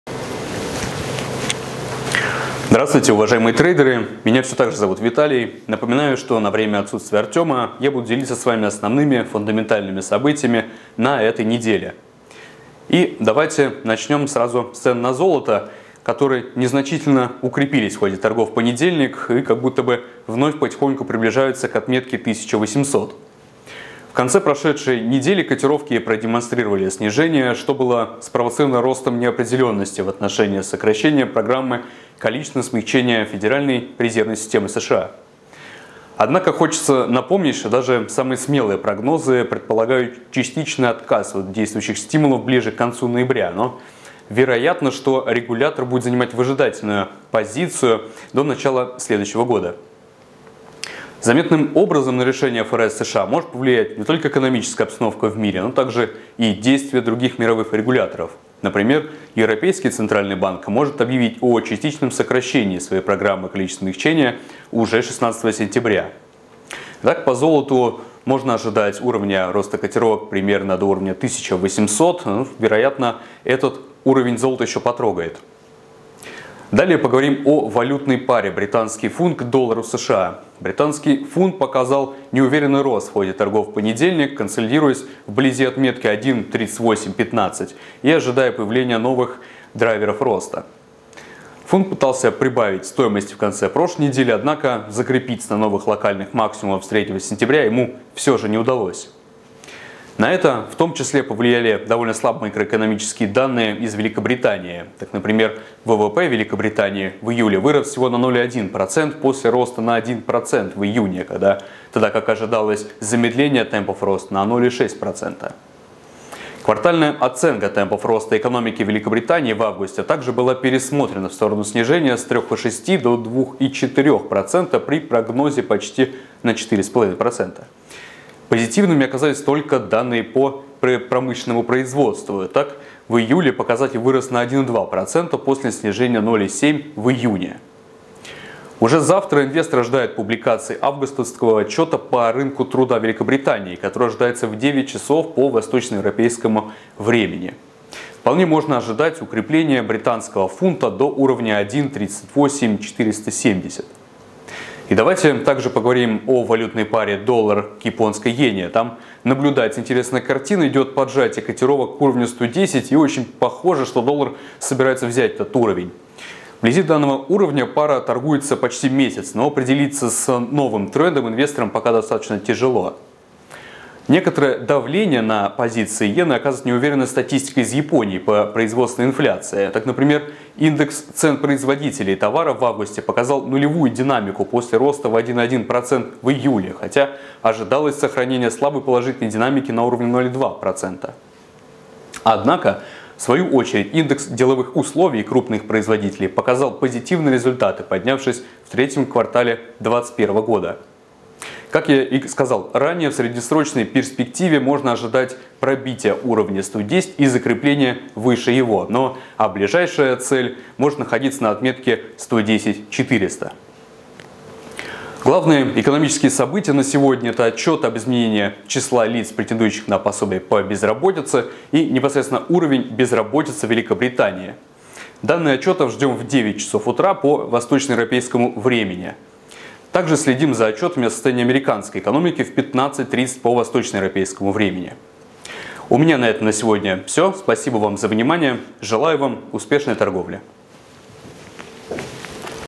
Цены на золото снова 1800? | AMarkets
О золоте, замедлении экономики Великобритании, американской и английской статистике, экономической неопределенности в Европе, о перспективах денежно-кредитной политики США и ЕС, техническом и фундаментальном анализе, а также о ценовых ориентирах по парам золоту и валютным парам GBP/USD, USD/JPY.